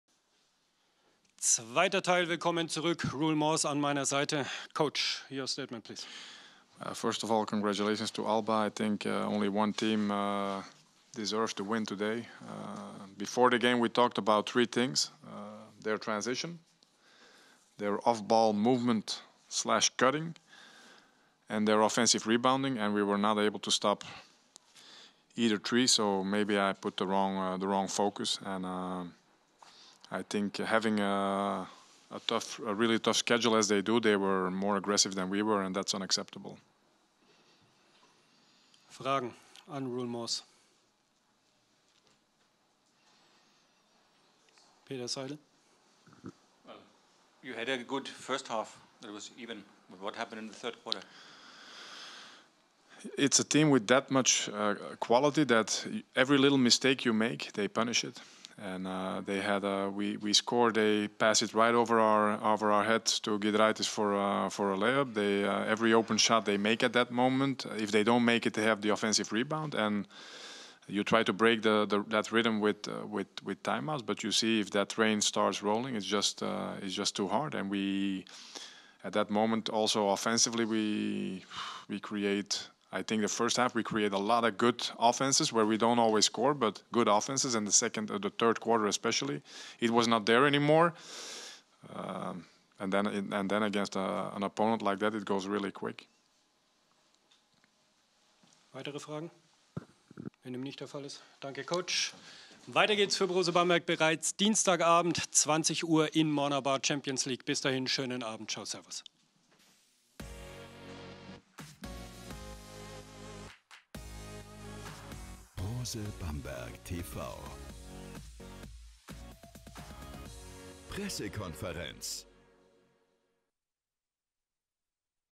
Pressekonferenz Brose Bamberg vs. ALBA BERLIN 66:82
Roel Moors schildert seine Sicht der Dinge zum Halbfinalaus.